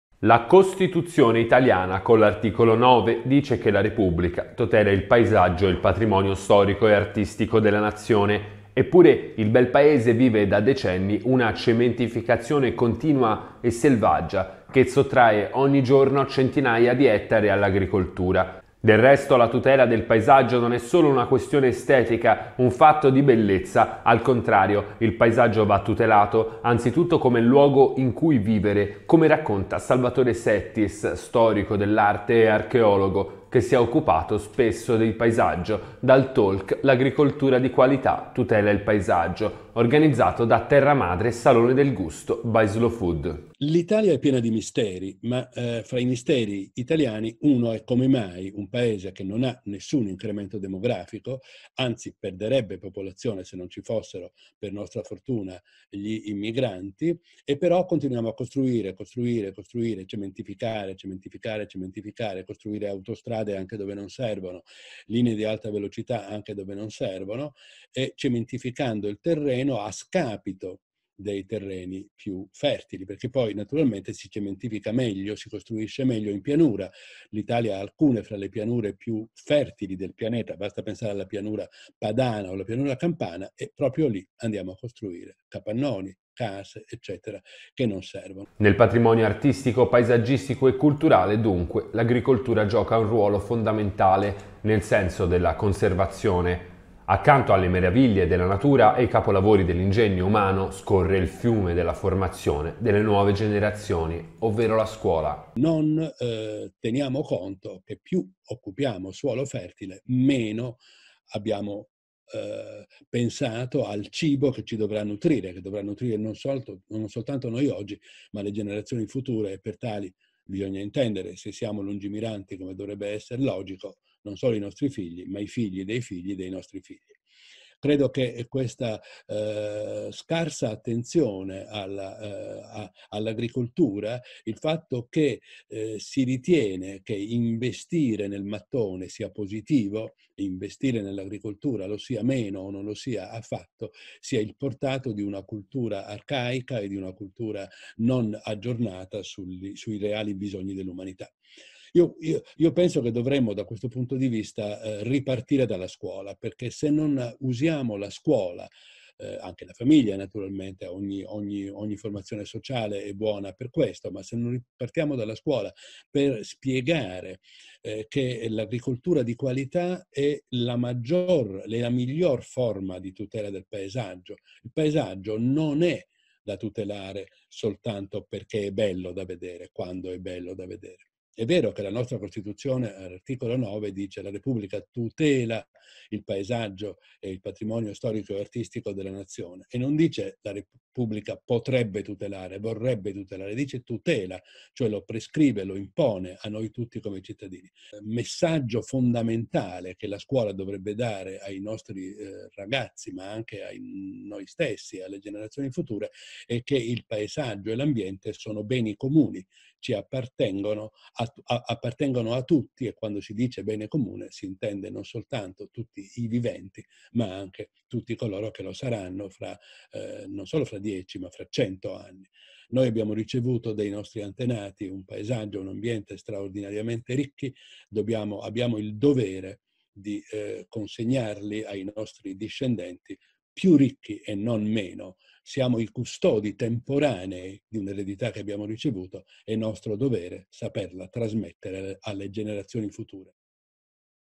Settis: “Siamo custodi temporanei di un’eredità, nostro dovere trasmetterla alle generazioni future”
“Come mai un Paese che non ha alcun incremento demografico, anzi perderebbe popolazione se non ci fossero, per nostra fortuna, gli immigranti, continuiamo a costruire e cementificare? A scapito dei terreni fertili, anche perché si costruisce meglio in pianura, e l’Italia ha alcune delle pianure più fertili del pianeta, dalla pianura Padana alla pianura Campana, dove costruiamo capannoni e case che non servono. Tutto ciò ha una spiegazione molto complessa: in sostanza, da una cultura molto arcaica il mattone è ritenuto una forma di investimento per surgelare un capitale accumulato, comunque lo si sia accumulato. Sia onestamente che dalle attività criminali”. Così, Salvatore Settis, dal food talk “L’agricoltura di qualità tutela il paesaggio”, organizzato nei giorni scorsi da Terra Madre - Salone del Gusto di Slow Food.